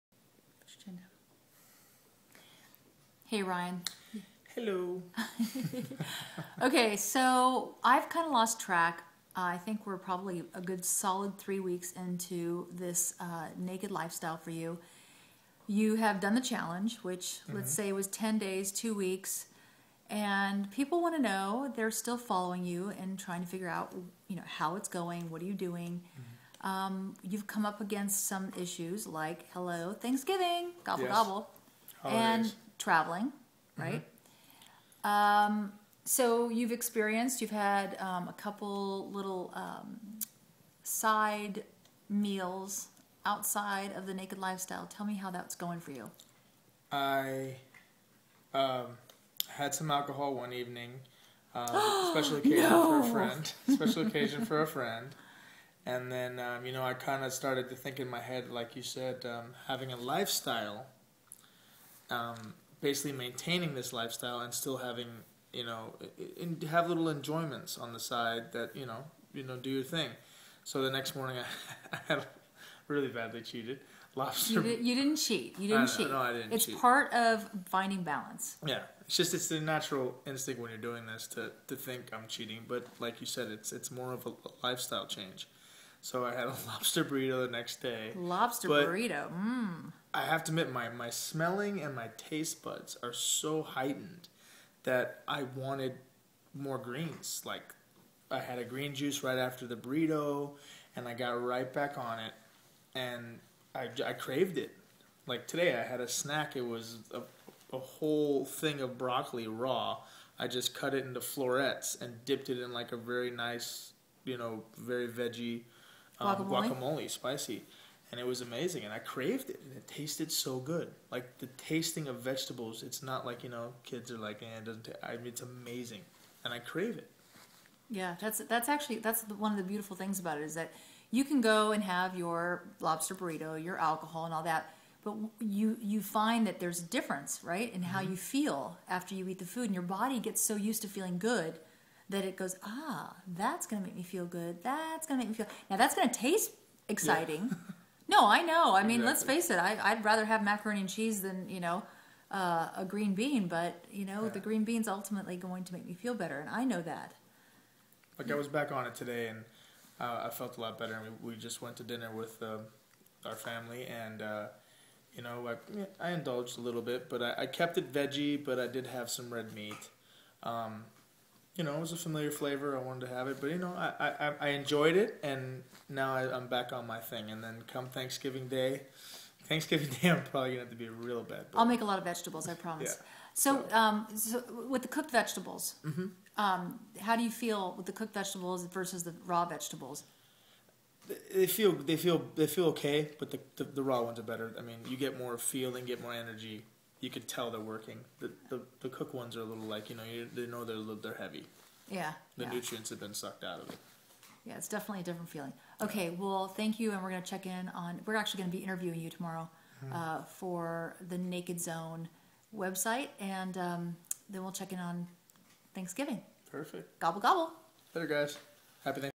Day 22- Farrowtone The Naked Lifestyle
Ryan is navigating through the holiday temptations- discovering that the "feel good" foods (greens) are what he turns to for comfort.